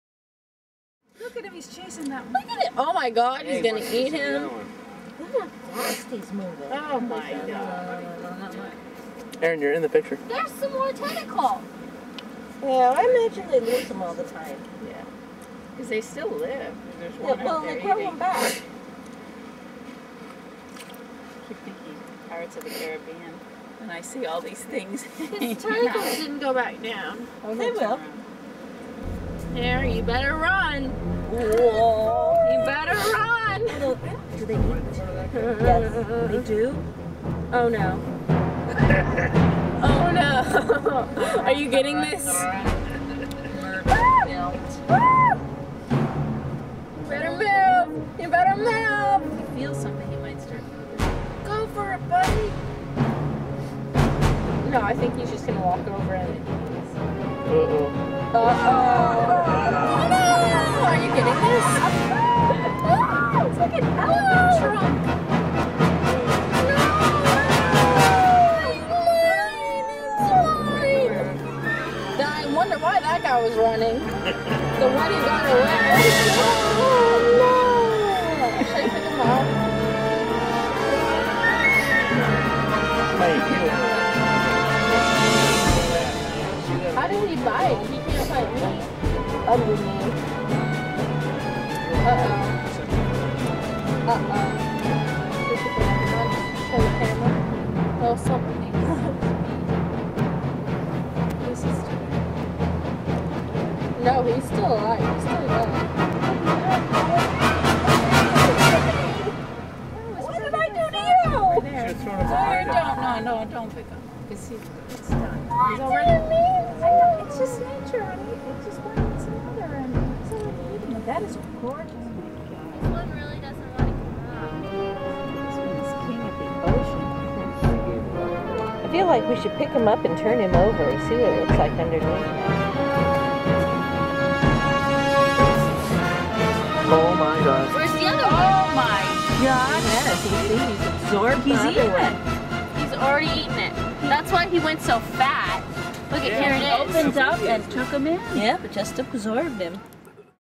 Cannibalistic Sea star
Filmed in British Columbia in some tide pools, a big sea star eats a little one!